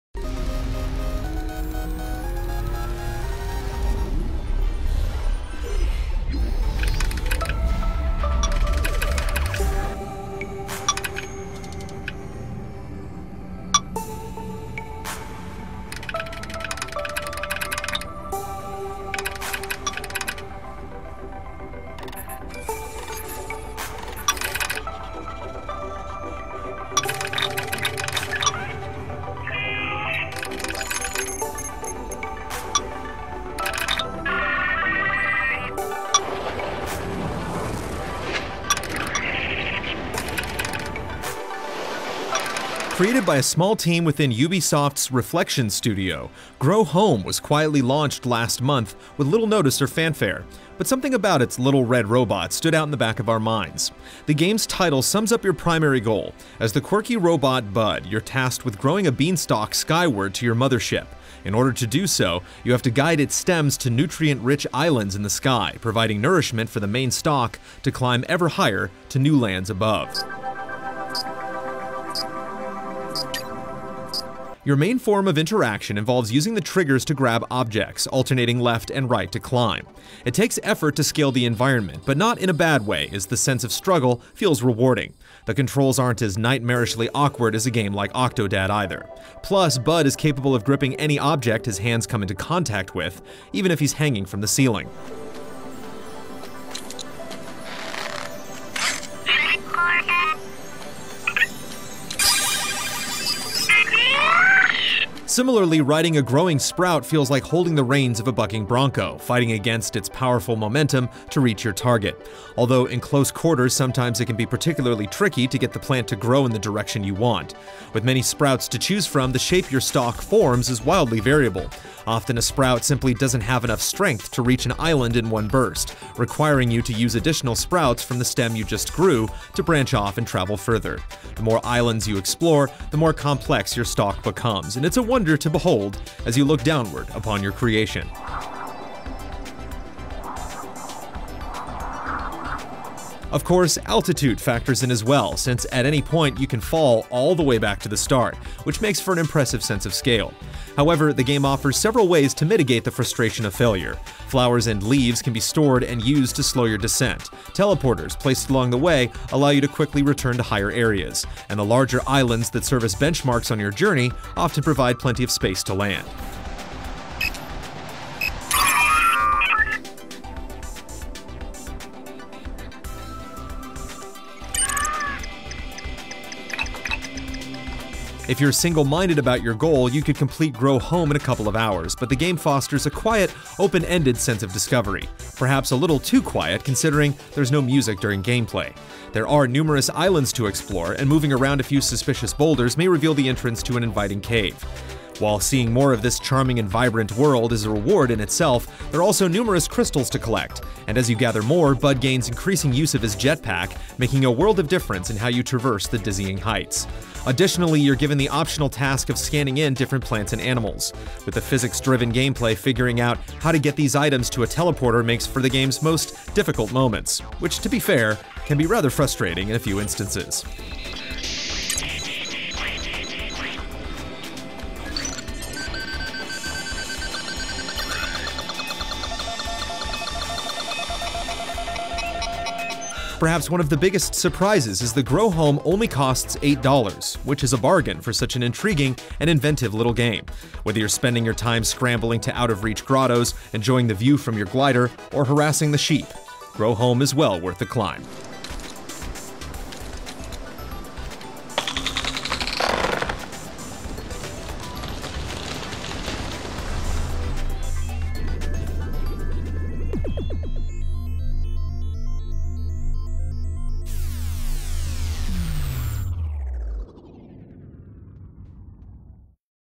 Grow Home-Review-GameTrailers
Sprout skyward and levitate sheep in this surprise new adventure from Ubisoft.